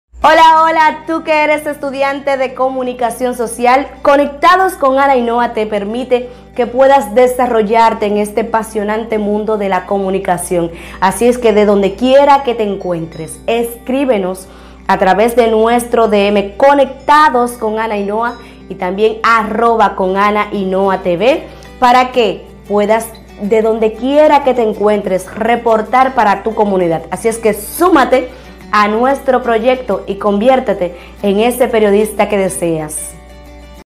conecta2 abre las puertas a los estudiantes de comunicación
El el multimedio conecta2 abre sus puertas a los estudiantes para que se desarrollen en la comunicacion.